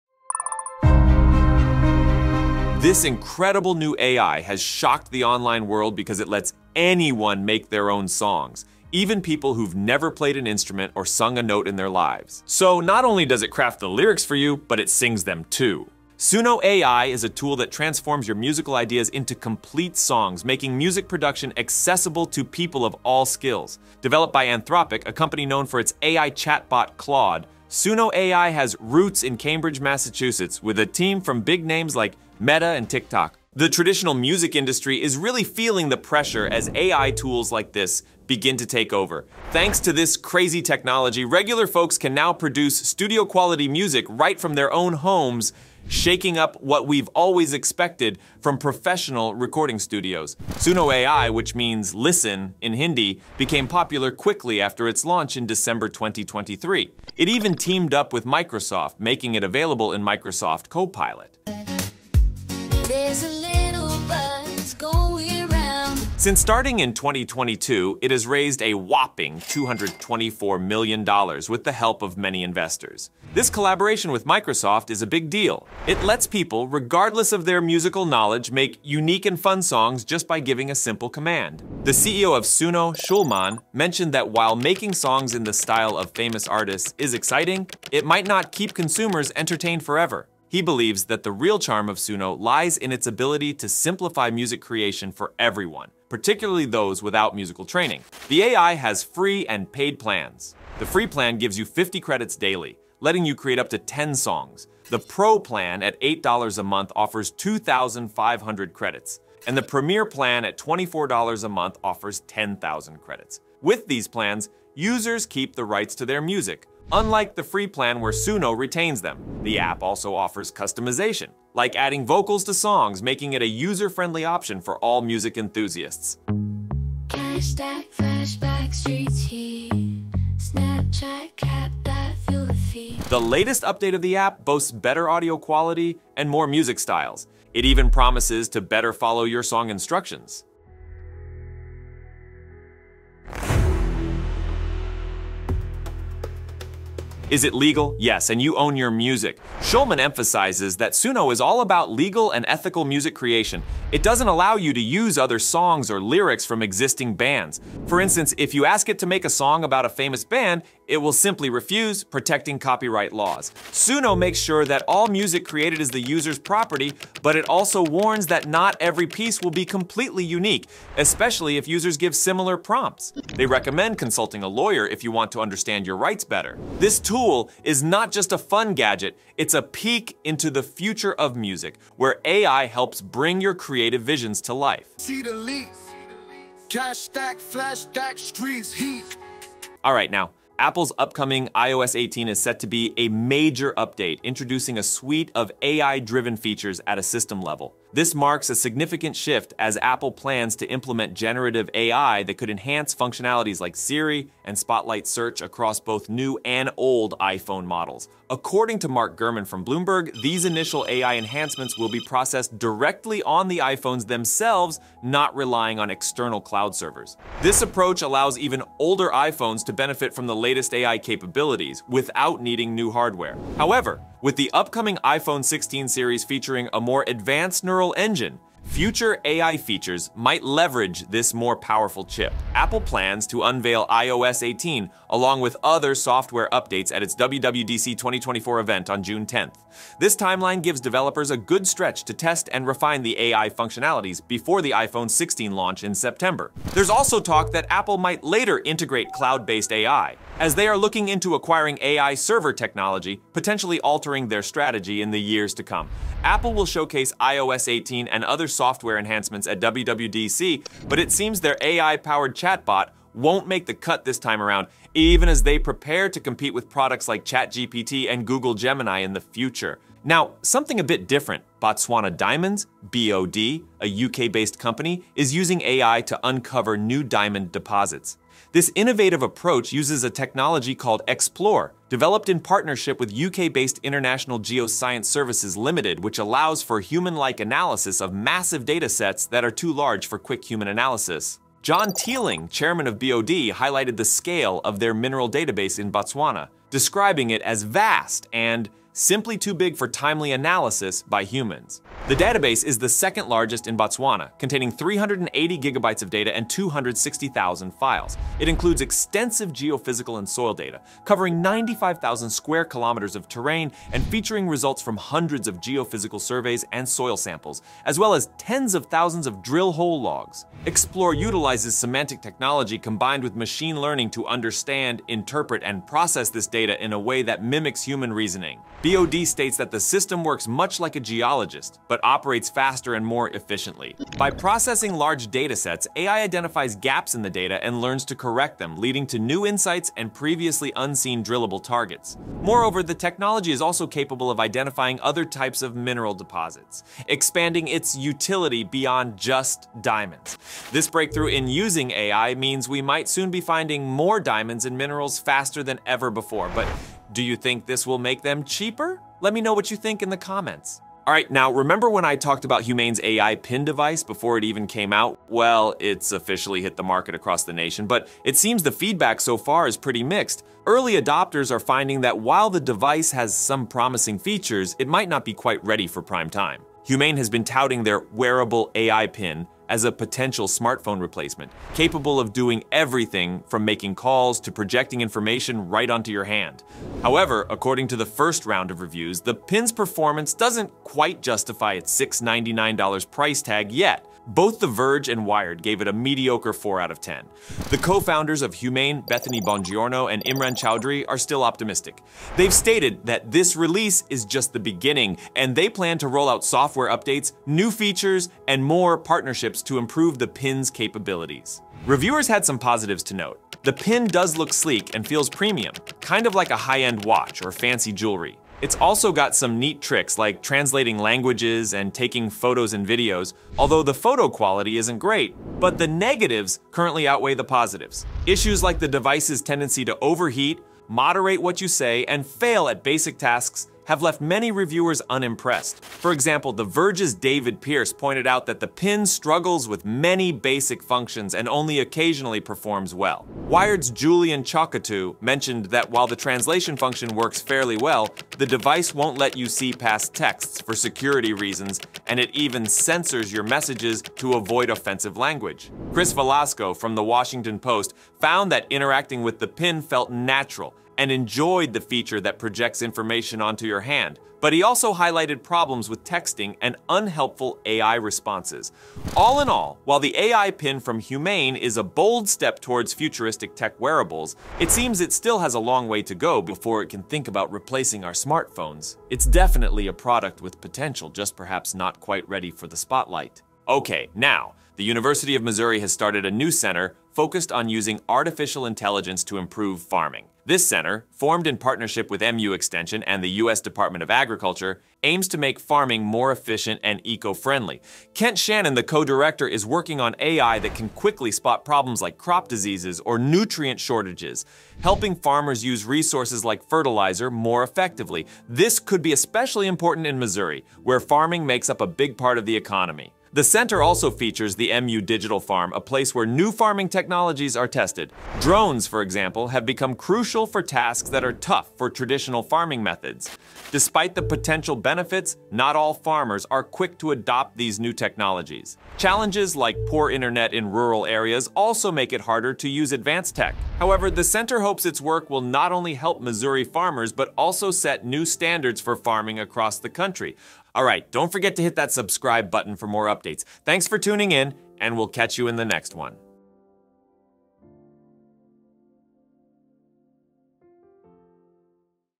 Anthropic's SUNO AI Breaks the Internet, iPhone On-Device AI Features, Humane AI Pin Reviews & More!
Description: Suno AI by Anthropic enables anyone to create songs without musical experience, featuring integration with Microsoft and various subscription plans. Apple's upcoming iOS 18 will introduce AI-driven features to enhance functionalities across iPhone models, processed directly on the devices. Additionally, innovative uses of AI by Botswana Diamonds and the University of Missouri are optimizing diamond mining and agriculture, focusing on efficiency and sustainability.